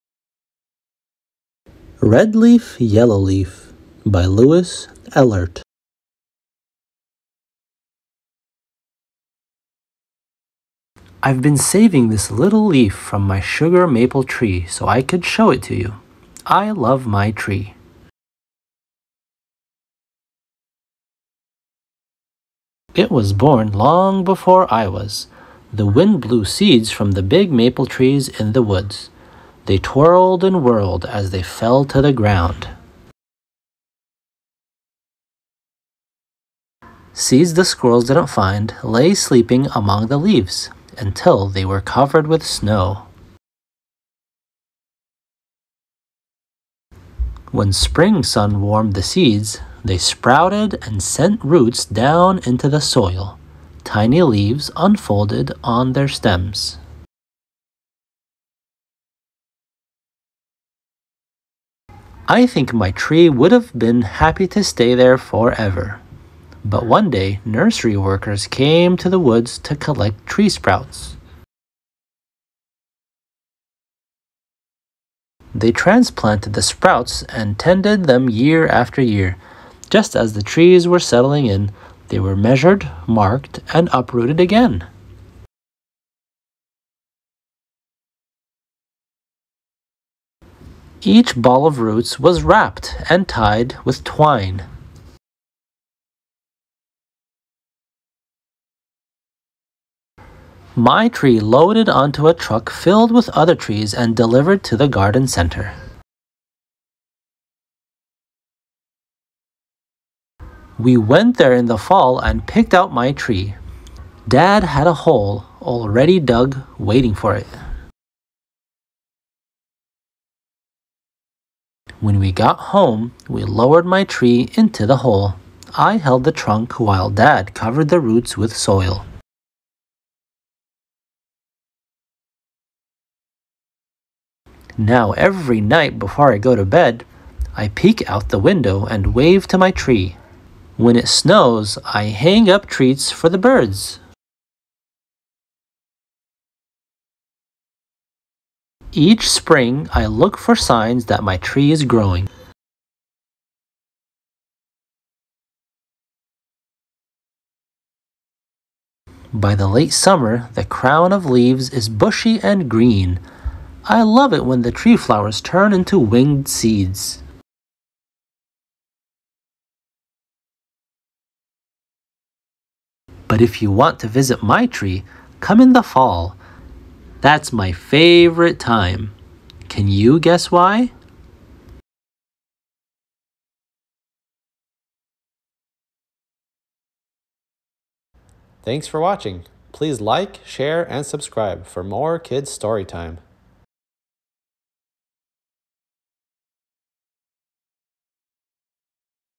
Red Leaf, Yellow Leaf by Lois Ehlert - Story Time | READ ALOUD
In this innovative and colorful work of early nonfiction from Caldecott Honor–winning author Lois Ehlert, little ones are introduced to facts about trees.

Ehlert blends bold graphics, vibrant colors, and precise details in her watercolor collage alongside found objects like seeds, fabric, wire, and roots.

A special glossary explains how roots absorb nutrients, what photosynthesis is, how sap circulates, how to make a bird treat, and other facts about trees. Children and adults will feel inspired to take a closer look at nature in their own backyards.